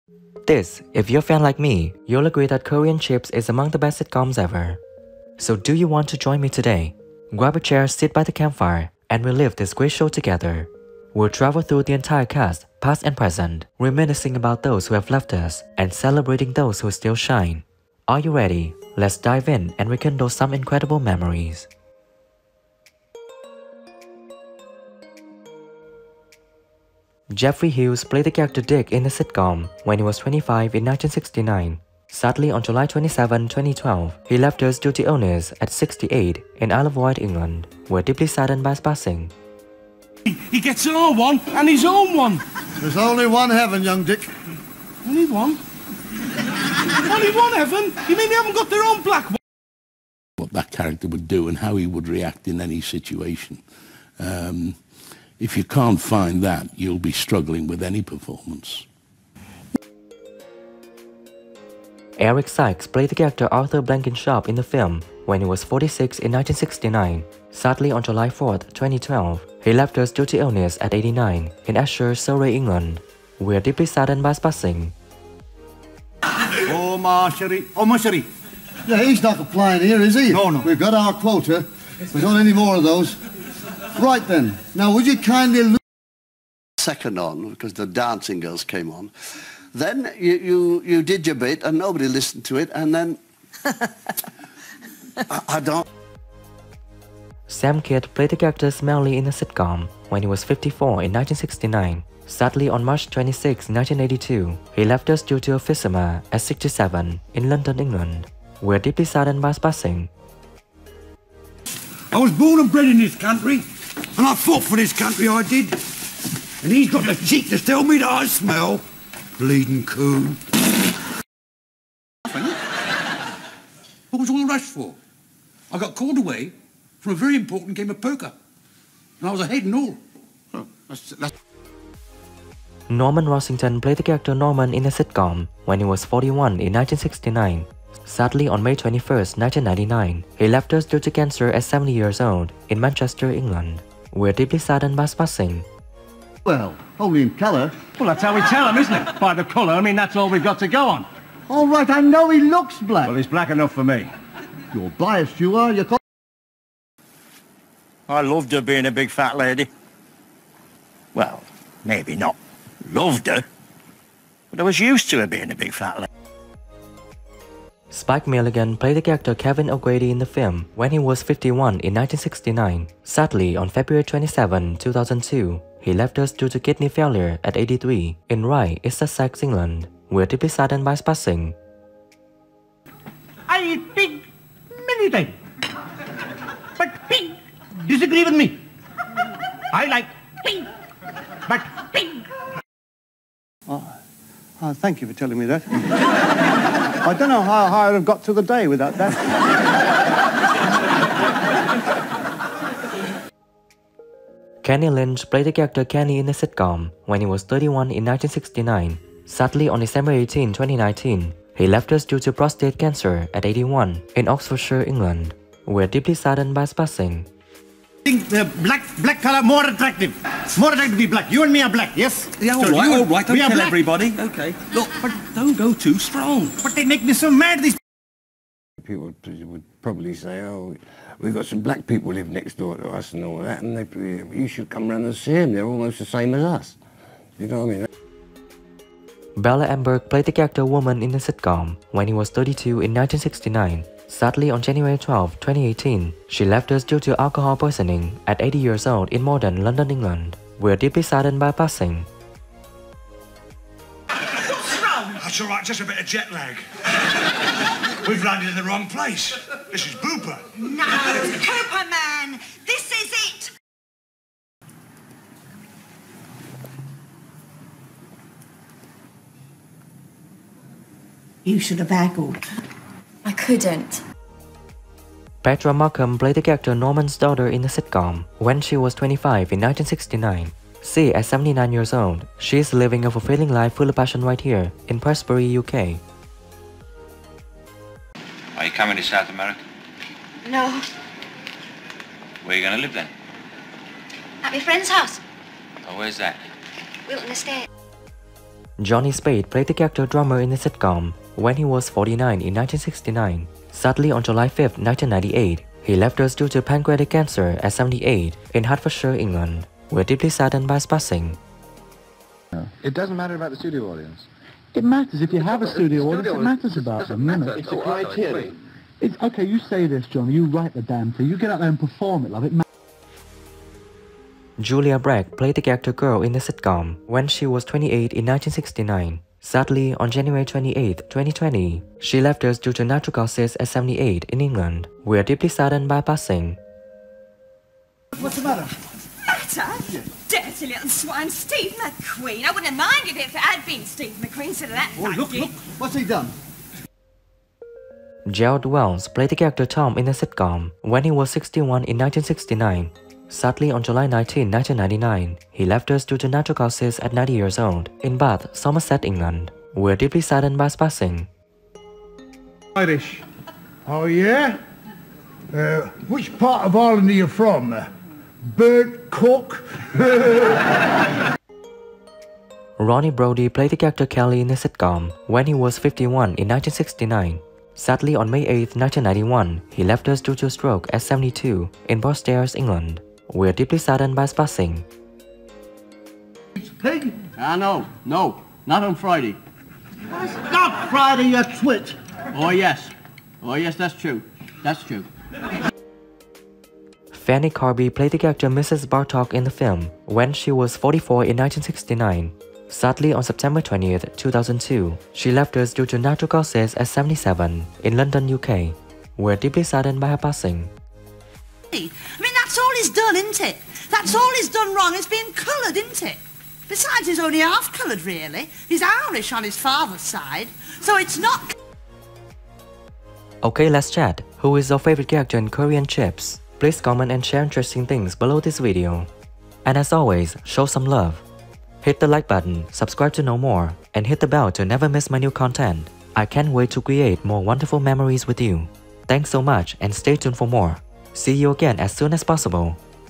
CURRY AND CHIPS 1969 Cast Then and Now 2024, All Cast Tragically Passed Away!
Curry and Chips is a British television sitcom broadcast in 1969 which was produced by London Weekend Television for the ITV network.

Set on a factory floor of 'Lillicrap Ltd', it starred a browned up Spike Milligan as an Irishman of Pakistani heritage named Kevin O'Grady, who also featured in episode 7 of the fifth series of Speight's Till Death Us Do Part. It also featured Eric Sykes as the foreman, Norman Rossington as the shop steward, and other regulars were Kenny Lynch, and Sam Kydd. The series was written by Till Death Us Do Part writer Johnny Speight, but based on an idea by Milligan. The programme was cancelled for what some (including the ITA) considered racist humour.